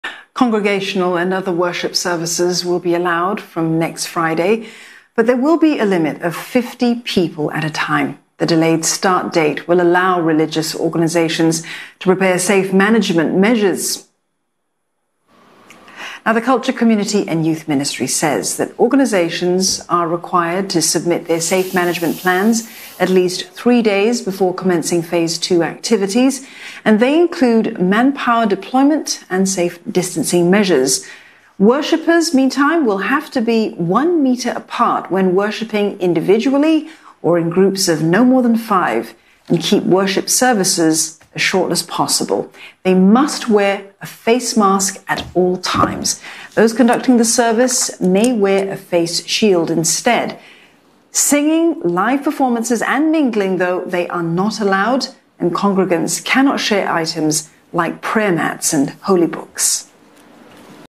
Places of worship to allow congregations of up to 50 people at a time
Congregational and other worship services will be allowed from Jun 26, but there will be a limit of 50 people at a time. The delayed start date will allow religious organisations to prepare safe management measures.